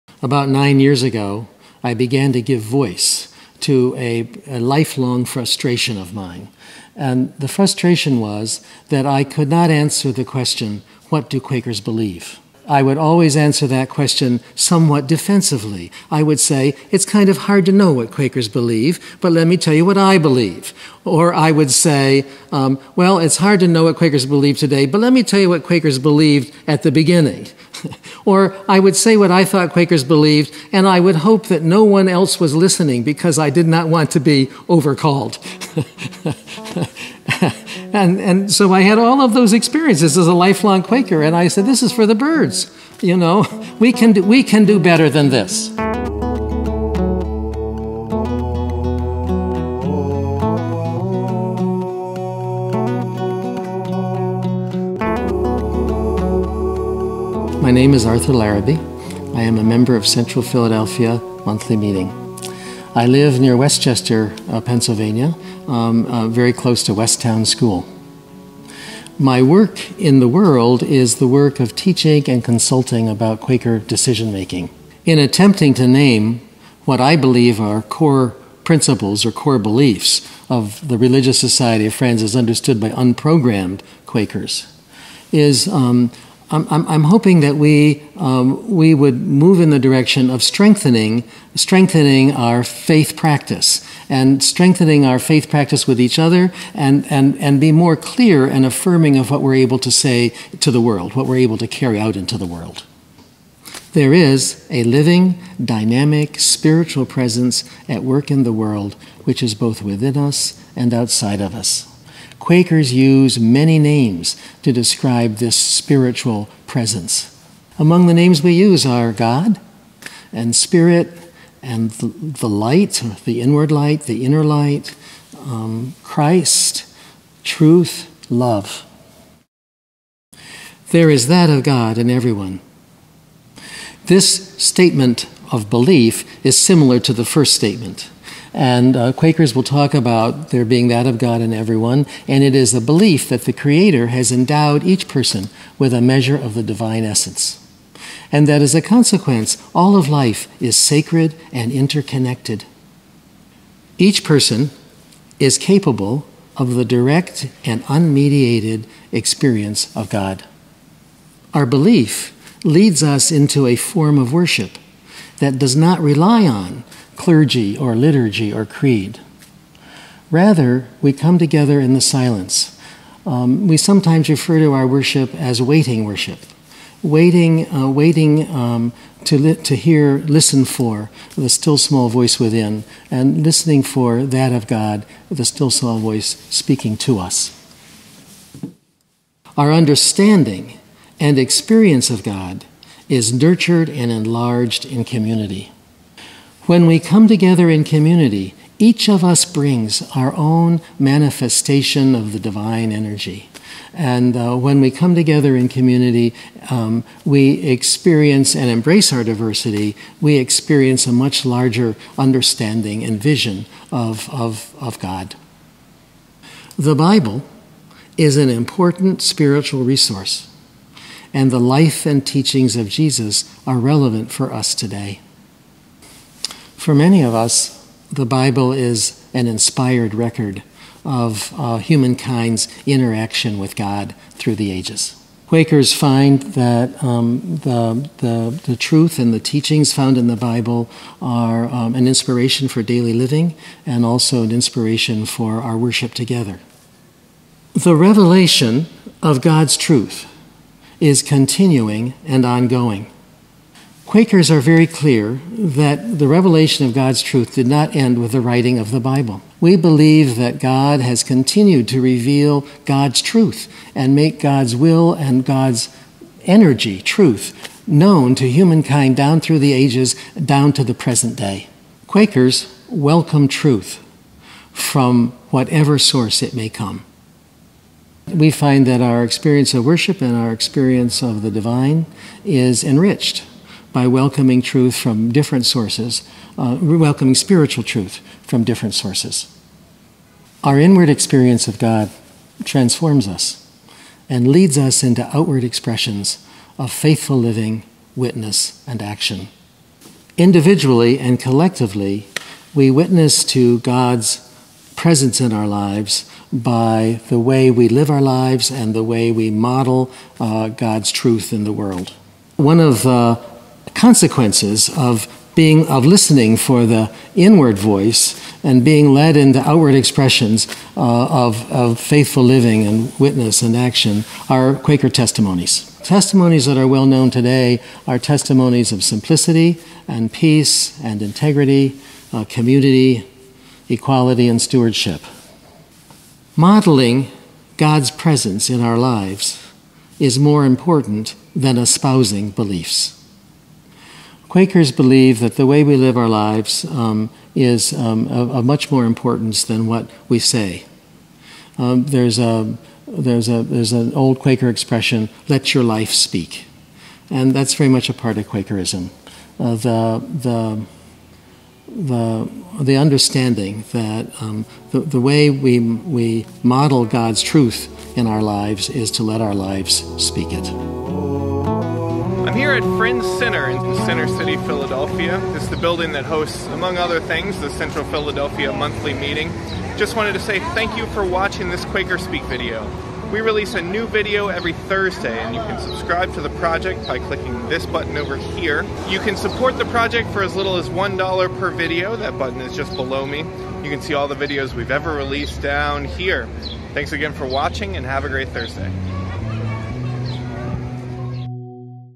9 Core Quaker Beliefs
As a lifelong Quaker, Arthur Larrabee was frustrated that he couldn’t answer the question, “What do Quakers believe?” So he set out to do just that.

FCE: deepening intimacy, finding peace, building community. Curious?

Learn about how Friends Fiduciary witnesses to Quaker values on Wall Street

Transcript:

About 9 years ago I began to give voice to a lifelong frustration of mine. The frustration was that I cannot answer the question “What do Quakers believe?” I would always answer the questions somewhat defensively. I would say, “it’s kind of hard to know what Quakers believe, but let me tell you what I believe.” Or I would say, “well, it’s hard to know what Quakers believe today but let me tell you what Quakers believed at the beginning.” Or I would say what I thought Quakers believed and I would hope that no one else was listening because I did not want to be overcalled.

And so I had all of those experiences as a lifelong Quaker and I said, “this is for the birds!” We can do better than this.

9 Core Quaker Beliefs

My name is Arthur Larrabee. I’m a member of Central Philadelphia Monthly Meeting. I live near West Chester, Pennsylvania, very close to Westtown School. My work in the world is the work of teaching and consulting about Quaker decision making.

In attempting to name what I believe are core principles, or core beliefs of the Religious Society of Friends as understood by unprogrammed Quakers, I’m hoping that we would move in the direction of strengthening our faith practice, and strengthening our faith practice with each other and be more clear and affirming of what we’re able to say to the world, what we’re able to carry out into the world.

1. There is a living, dynamic, spiritual presence at work in the world which is both within us and outside of us.

Quakers use many names to describe this spiritual presence. Among the names we use are God, spirit, the light, the inward light, the inner light, Christ, truth, love.

2. There is that of God in everyone.

This statement of belief is similar to the first statement, and Quakers will talk about there being that of God in everyone, and it is the belief that the creator has endowed each person with a measure of the divine essence, and that as a consequence, all of life is sacred and interconnected.

3. Each person is capable of the direct and unmediated experience of God.

Our belief leads us into a form of worship that does not rely on clergy or liturgy or creed. Rather, we come together in the silence. We sometimes refer to our worship as “waiting worship.” Waiting to hear—listen for—the still, small voice within, and listening for that of God—the still, small voice—speaking to us.

4. Our understanding and experience of God is nurtured and enlarged in community.
When we come together in community, each of us brings our own manifestation of the divine energy. When we come together in community, we experience and embrace our diversity; we experience a much larger understanding and vision of God.

5. The Bible is an important spiritual resource, and the life and teachings of Jesus are relevant for us today.

For many of us, the Bible is an inspired record of humankind’s interaction with God through the ages. Quakers find that the truth and the teachings found in the Bible are an inspiration for daily living and also an inspiration for our worship together.

6. The revelation of God’s truth is continuing and ongoing.

Quakers are very clear that the revelation of God’s truth did not end with the writing of the Bible. We believe that God has continued to reveal God’s truth and make God’s will and energy, truth—known to humankind down through the ages, down to the present day.

7. We welcome truth from whatever source it may come.

We find that our experience of worship and our experience of the Divine is enriched by welcoming truth from different sources. We welcome spiritual truth from different sources.

The views expressed in this video are of the speakers and do not necessarily reflect the views of Friends Journal or its collaborators.